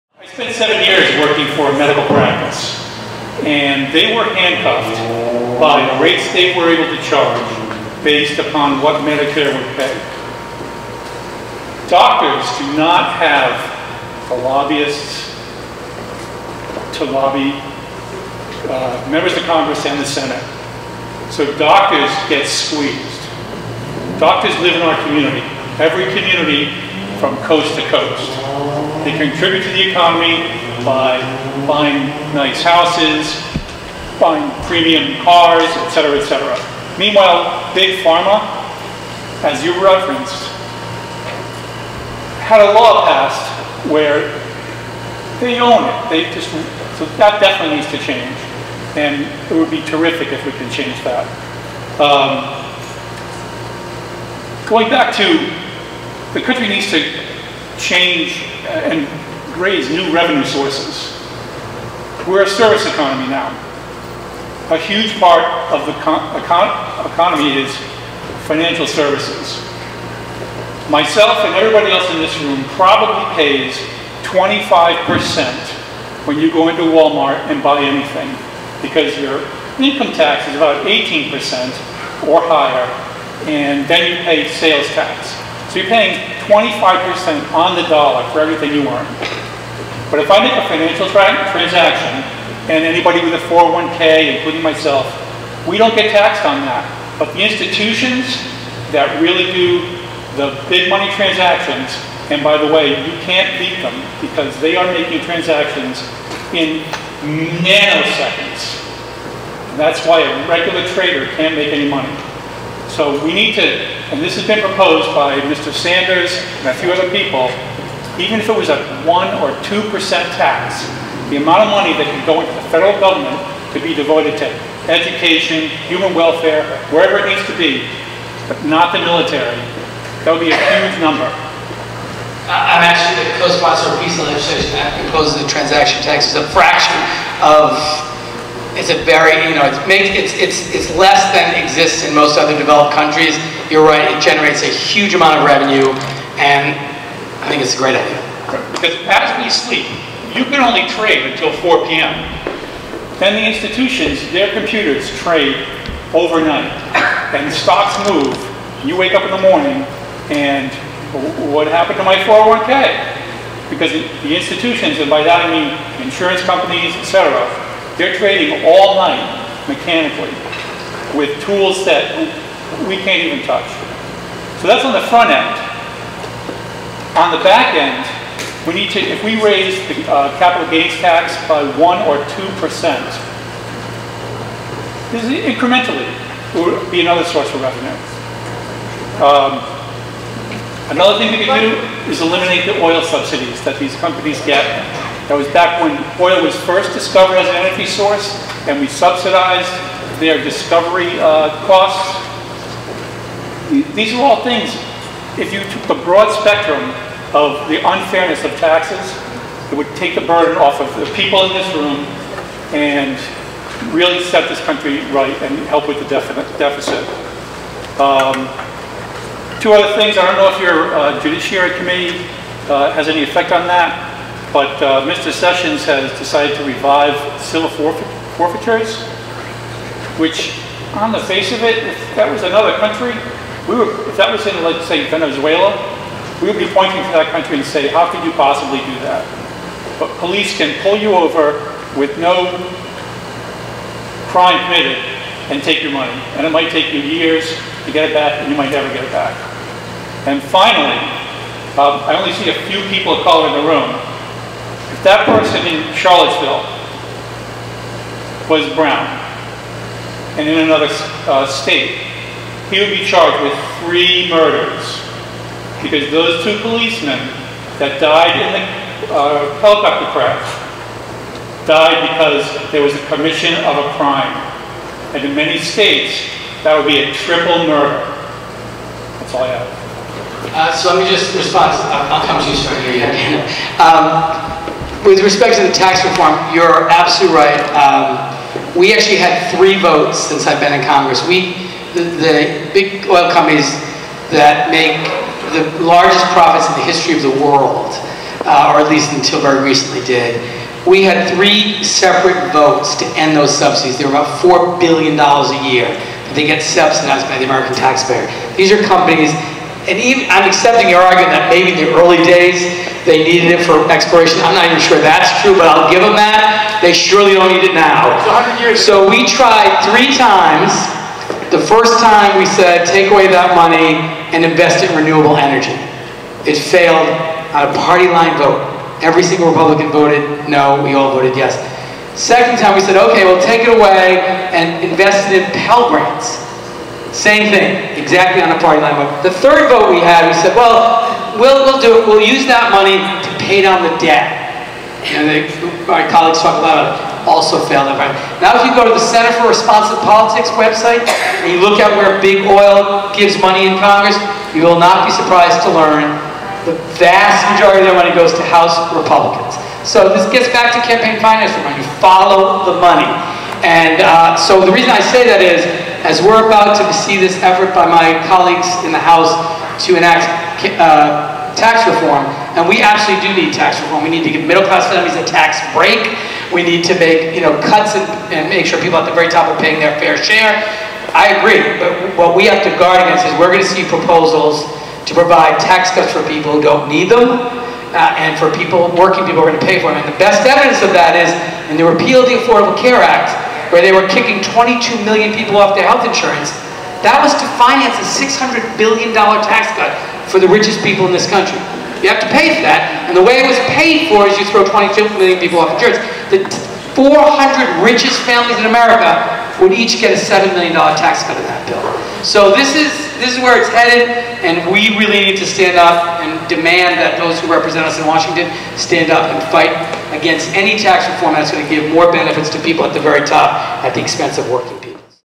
David Cicilline Exposes Tax Inequities at Town Hall
The video captures a town hall meeting held on August 16, 2017, in Cumberland, Rhode Island, featuring U.S. Representative David Cicilline. Cicilline addresses a range of topics, including tax reform, healthcare, and economic inequality, during his interaction with constituents. He discusses the disparity in taxation between everyday consumers and large financial institutions, advocating for a proposed financial transaction tax to generate revenue for public services. Cicilline also highlights efforts to end subsidies for oil companies, emphasizing the need to redirect funds toward renewable energy, Pell grants, and debt reduction. Additionally, he touches on issues of civil asset forfeiture and the impact of campaign finance on legislative decisions. The audience, predominantly white with few people of color, listens as Cicilline critiques policies that disproportionately benefit the wealthy at the expense of working-class Americans.